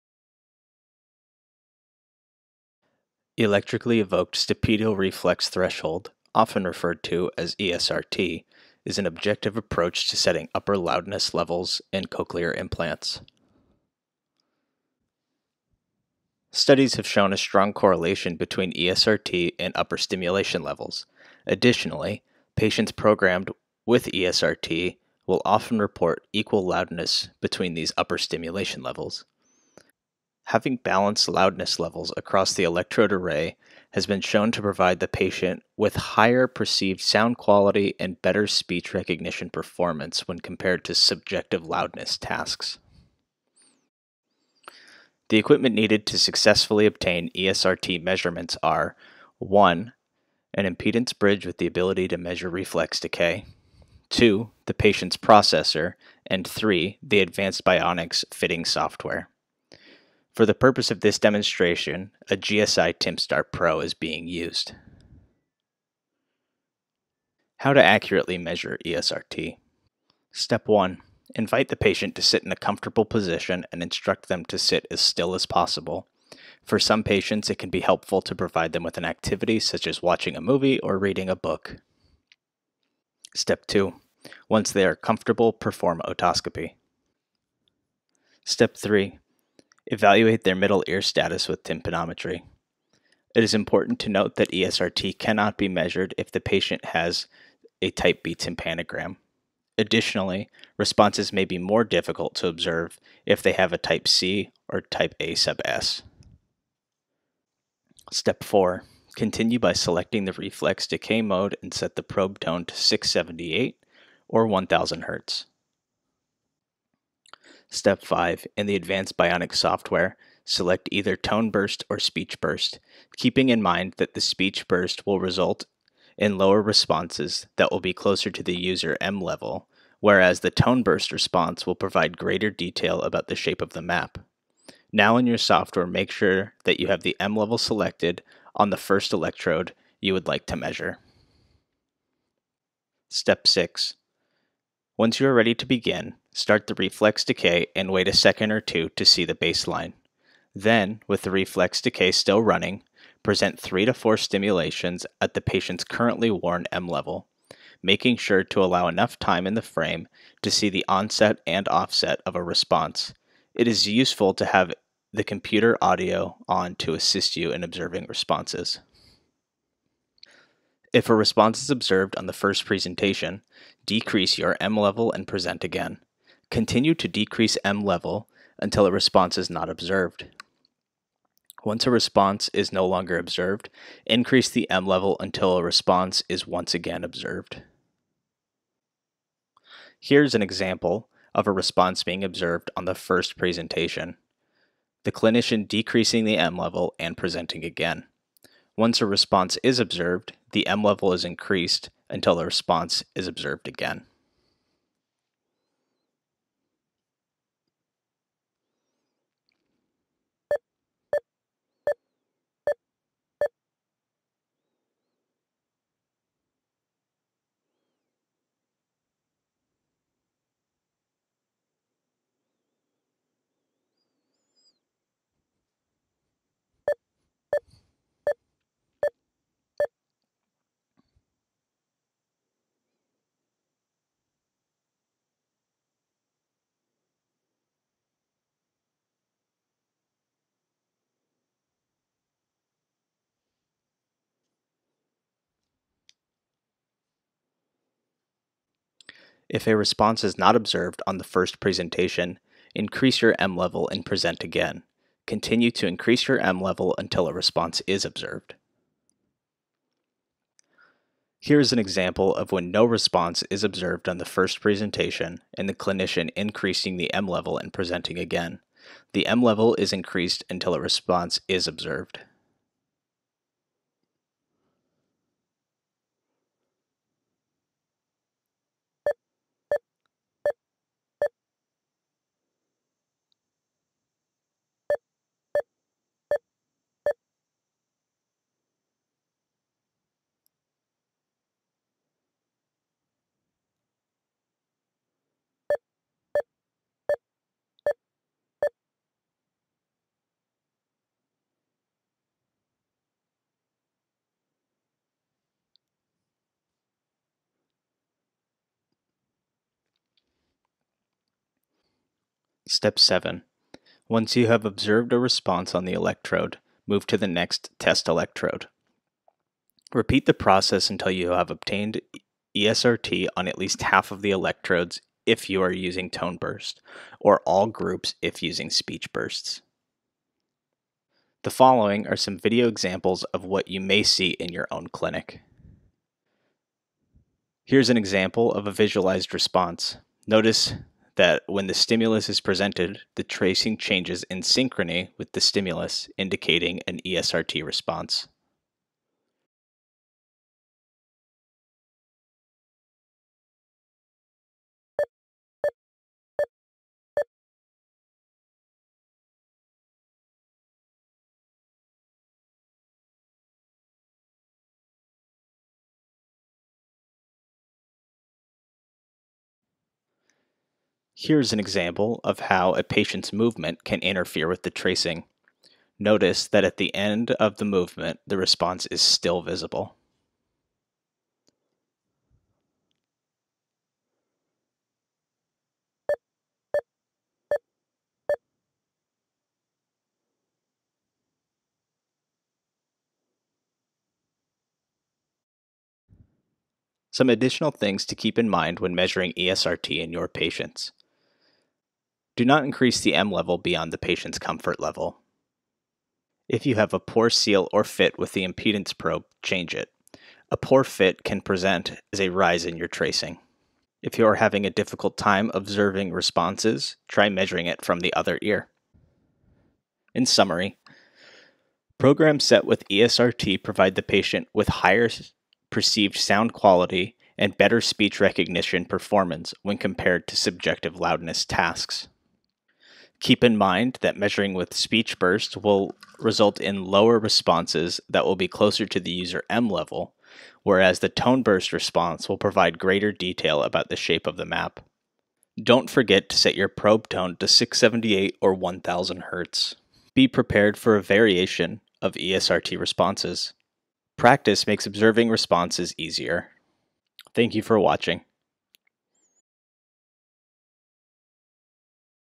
Measuring eSRT for Advanced Bionics Cochlear Implants
This video is a step by step process that I use for measuring eSRT, electrically evoked stapedial reflex thresholds, for Advanced Bionics cochlear implants.